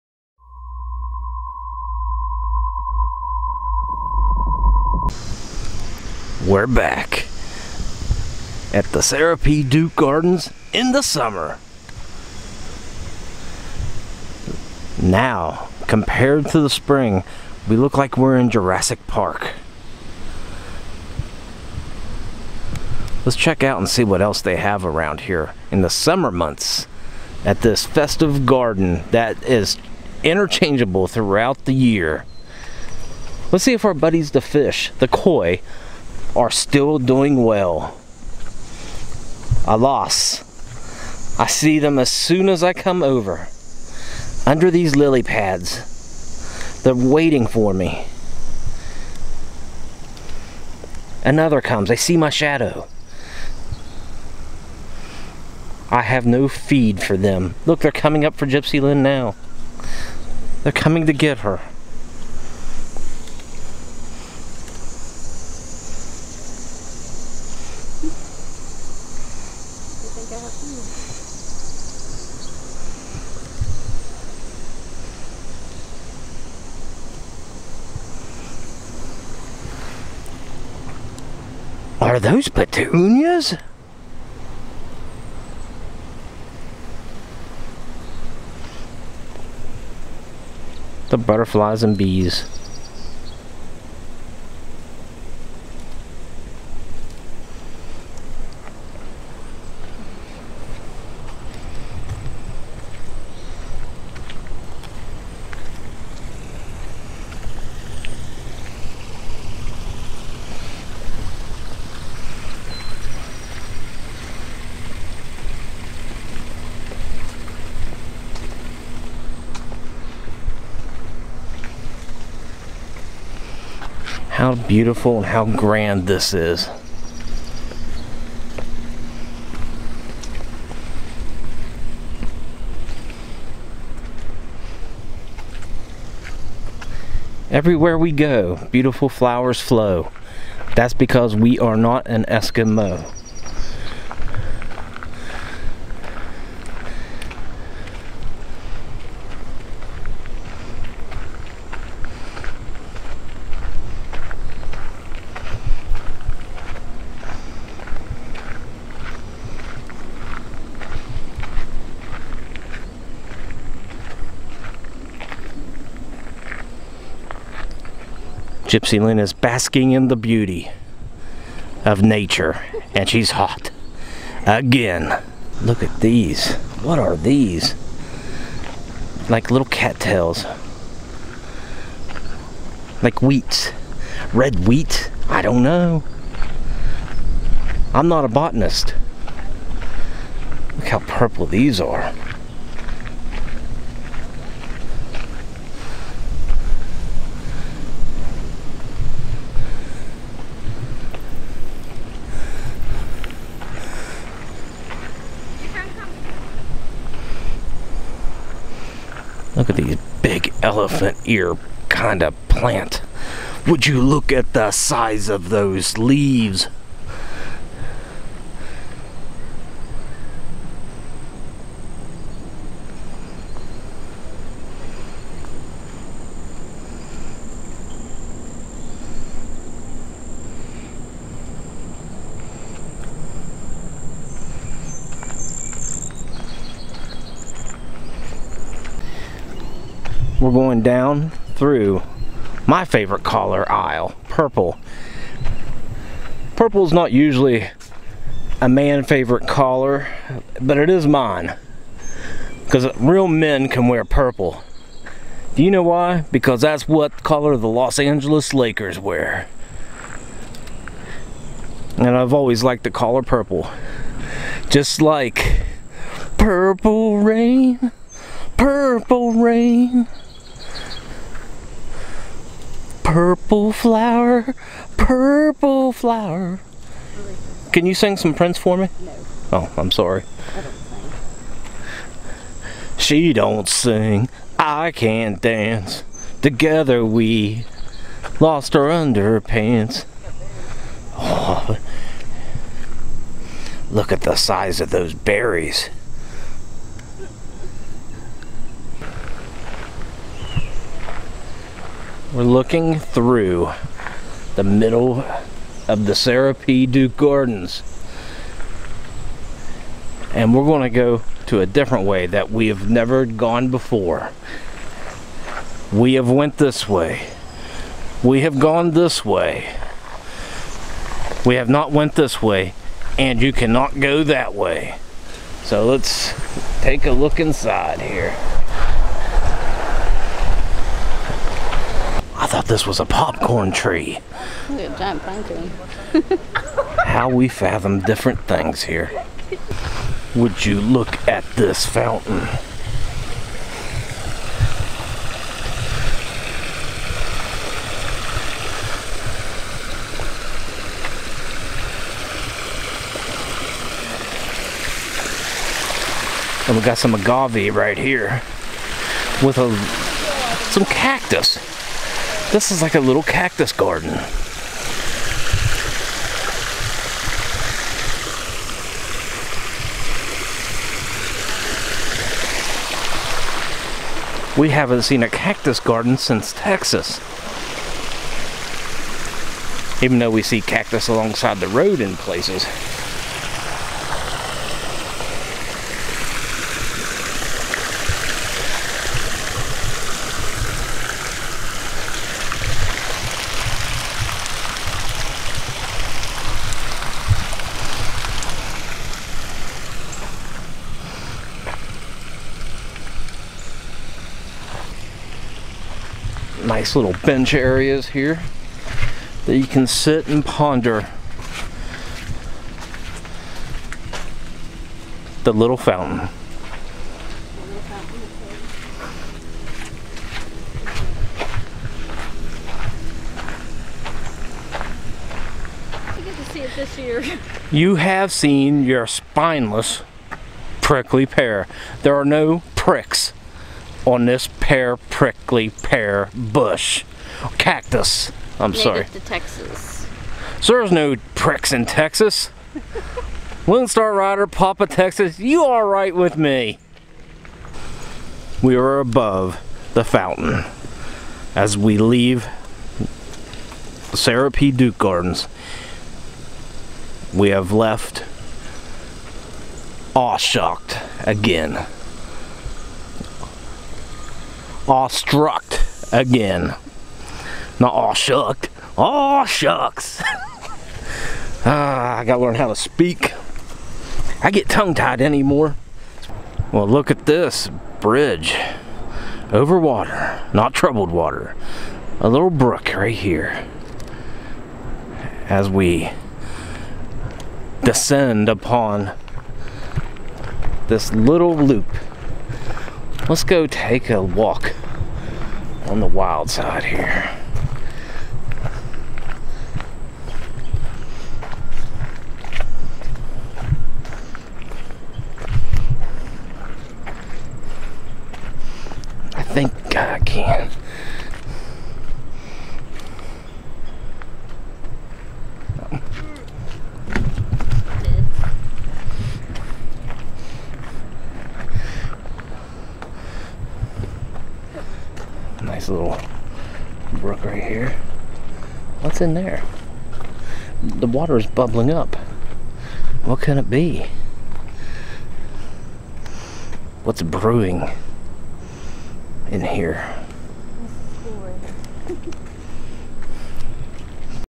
Sarah P. Duke Gardens In The Summer At Duke University (Durham, North Carolina)
We saw the flowers in the spring, now we get to check them out in the middle of summer.  A lot has changed since our last visit.  We explore some new areas of the gardens.

Thanks! Look forward to hearing from you.